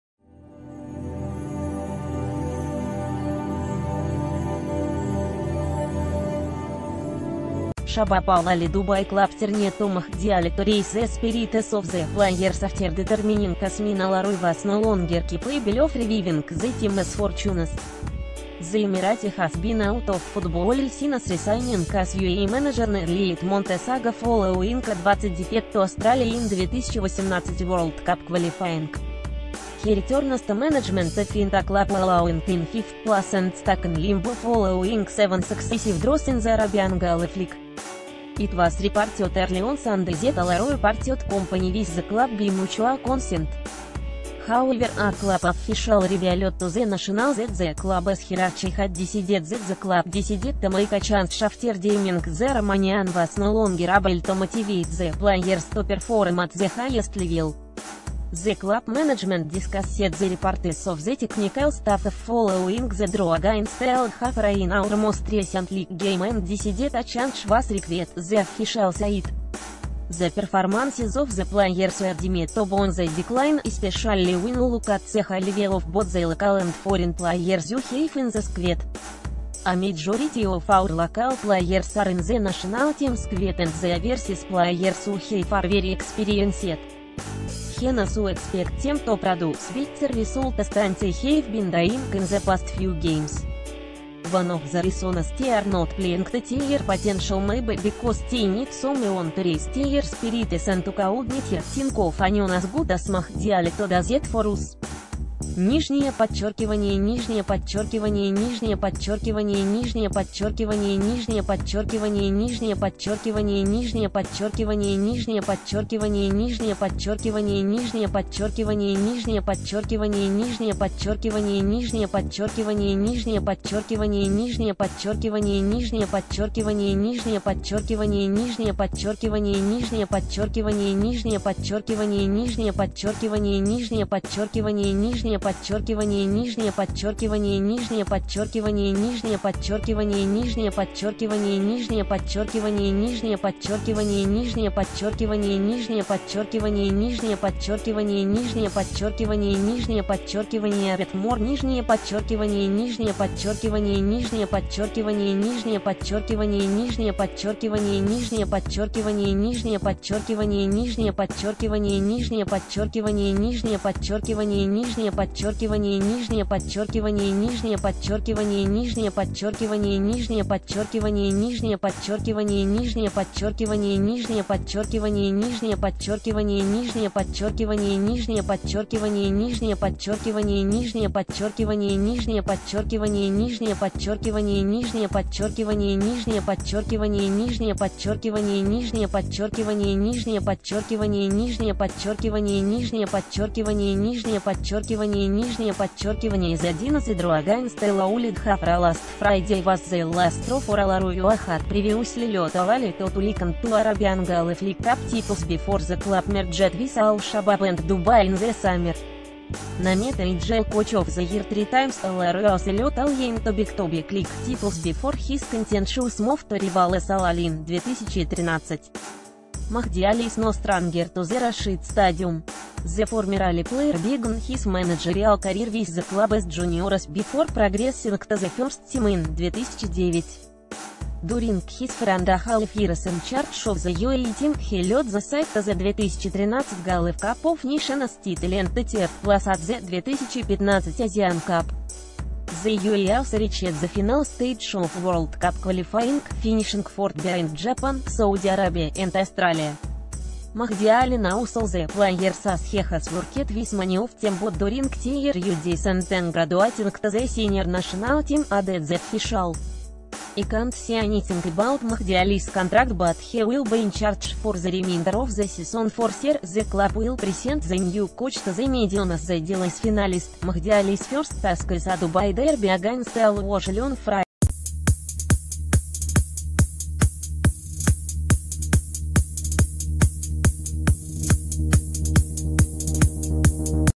Mahdi ali hired to 'raise the spirits' of struggling shabab al ahli dubai players, says officialBy
Mahdi ali hired to 'raise the spirits' of struggling shabab al ahli dubai players, says officialBy Channel TNC Sports
►* Fanpage:
►* Google Plus :